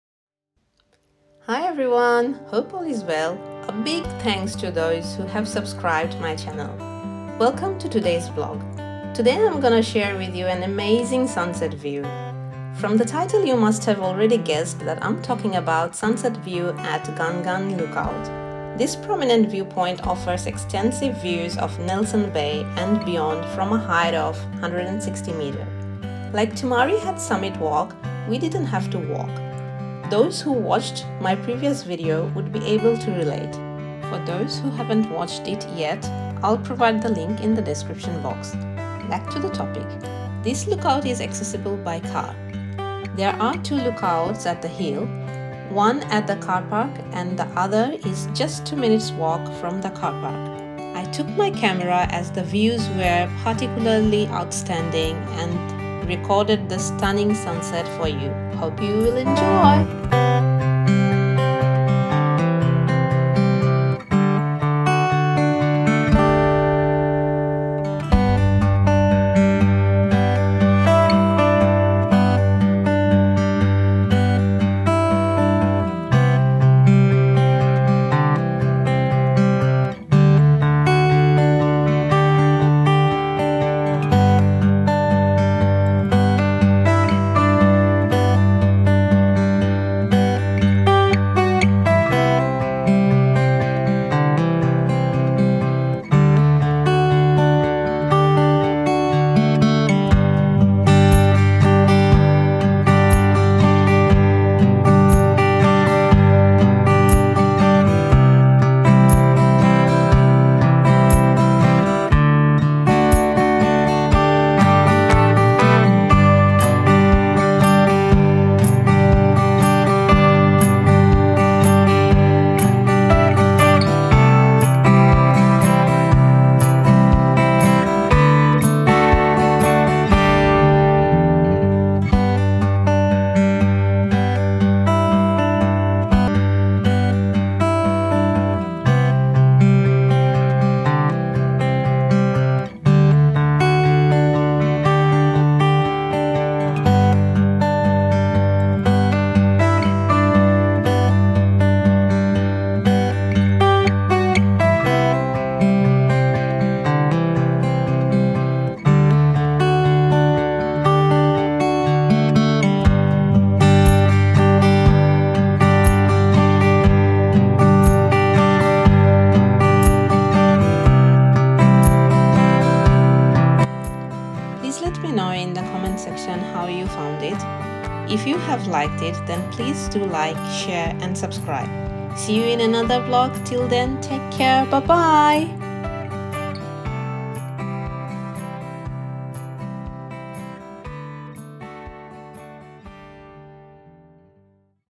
Sunset at Gan Gan Lookout । Nelson Bay । Port Stephens । NSW । Australia
Hi everyone! Hope all is well. Welcome to today’s vlog. Today I’m gonna share with you an amazing sunset view. From the title you must have already guessed that I’m talking about sunset view at Gan Gan lookout. This prominent viewpoint offers extensive views of Nelson Bay & beyond from a height of 160m. Like Tomaree Head Summit Walk we didn’t have to walk. Those who watched my previous video would be able to relate. For those who haven’t watched it yet, can watch it by clicking the

Back to the topic This lookout is accessible by car. There are 2 lookouts at the hill: one at the car park and the other is just 2 mins walk from the car park. I took my camera as the views were particularly outstanding and recorded the stunning sunset for you. Hope you’ll enjoy.

See you in another vlog. Till then take care. Bye bye!